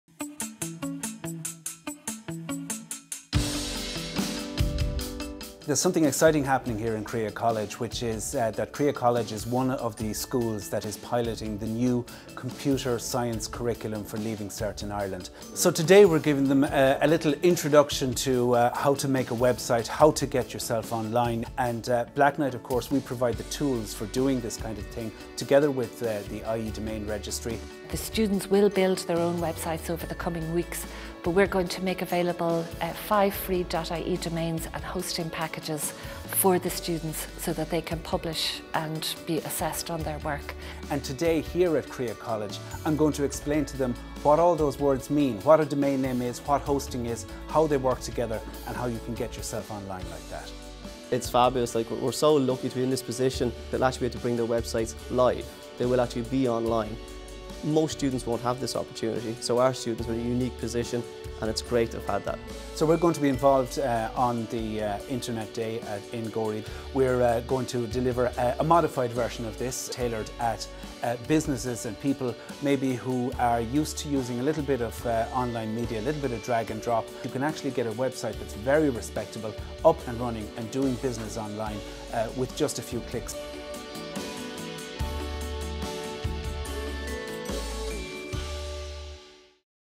Creagh College | Internet Day 2018 | IE Domain Registry
As part of Internet Day 2018 and celebrating Gorey as the Digital Town, Conn Ó Muíneacháin from Blacknight Internet Solutions and our Customer Operations Manager, Oonagh McCutcheon tell us about the recent web training provided to students in Creagh College and also the .ie domains and hosting packages that will be made available to five students so their websites will be available online. Tony McGrath from Creagh College discusses the benefits of this for his students.

To mark Internet Day 2018, IE Domain Registry has launched the ‘Digital Town’ initiative which will highlight the benefits and possibilities of the internet and celebrate the digital achievements of a local town.